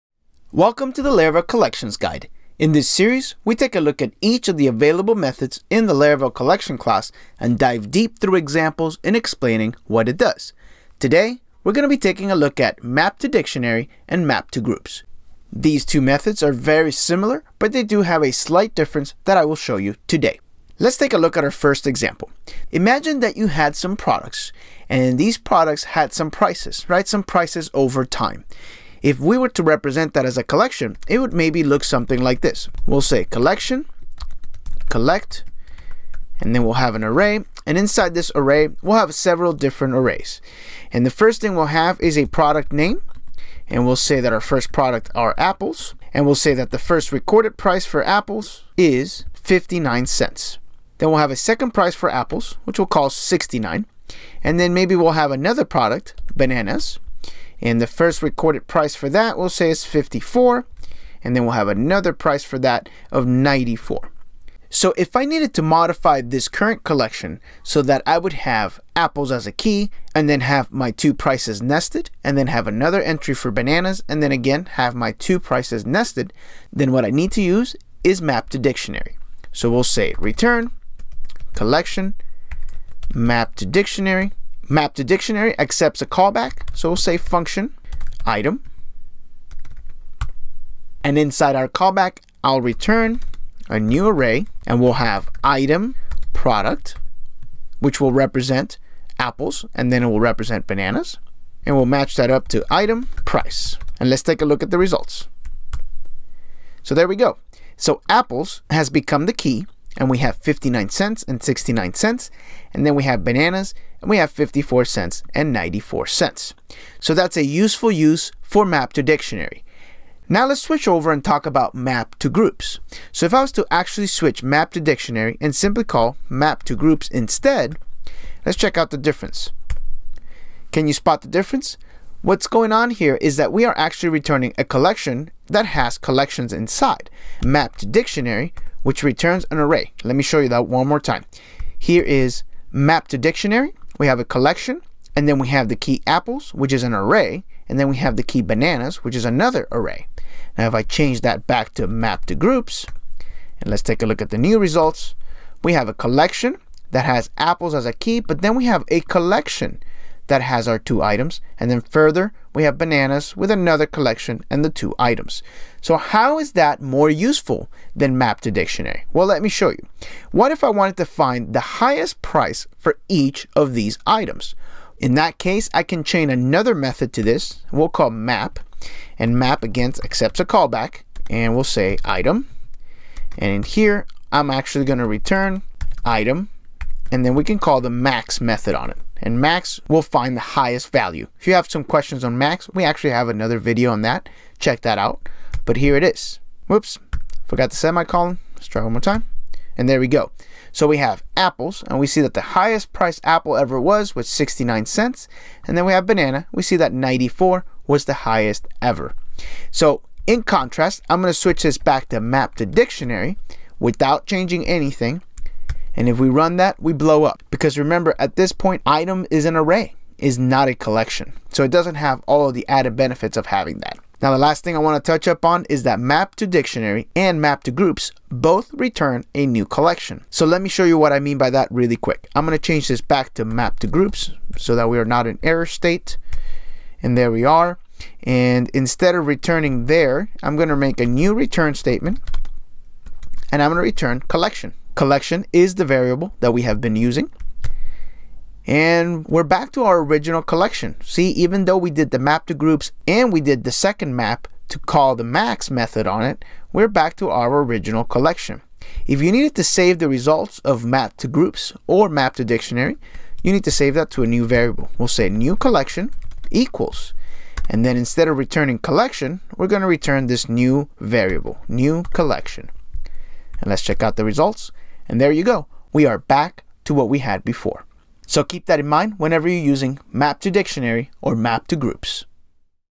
mapToDictionary mapToGroups | Laravel Collections
In this episode, we are tackling mapToDictionary and the very similar mapToGroups. Watch this video to see the difference between those two similar methods.

In this series, we take a look at each of the available methods in the Laravel Collection class and dive deep through examples in explaining what it does.